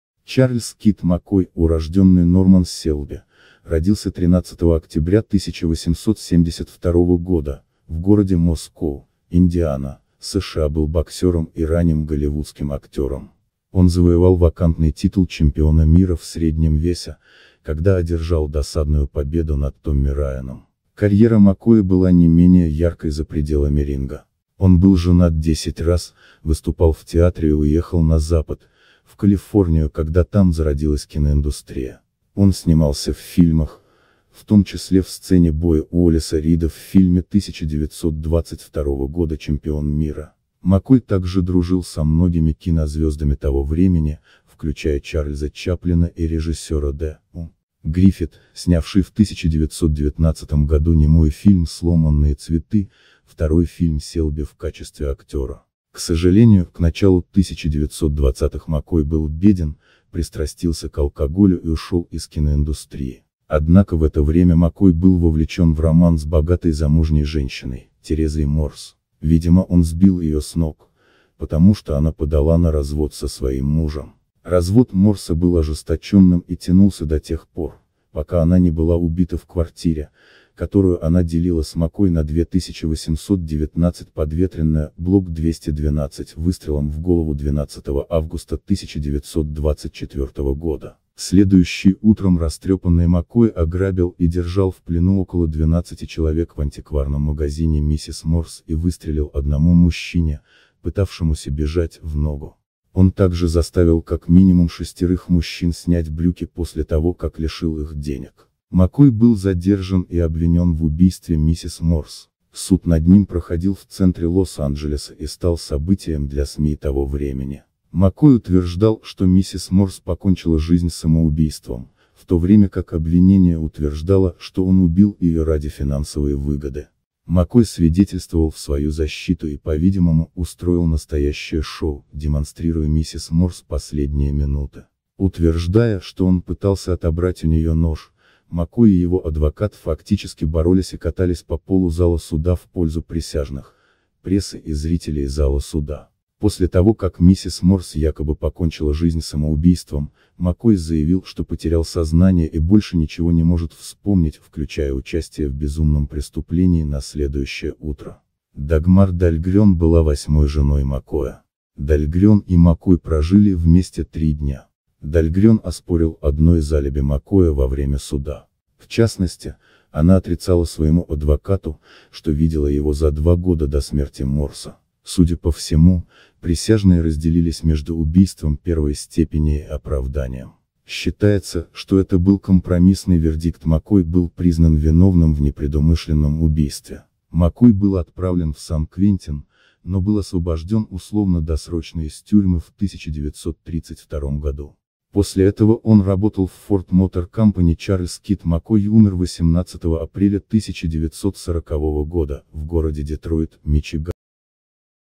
Чарльз «Кид» Маккой – биография и жизнь боксера с америки
Чарльз «Кид» Маккой, урожденный Норман Селби, был американским боксером и ранним голливудским актером. Он завоевал вакантный титул чемпиона мира в среднем весе, когда одержал досадную победу над Томми Райаном нокаутом в 15-м раунде.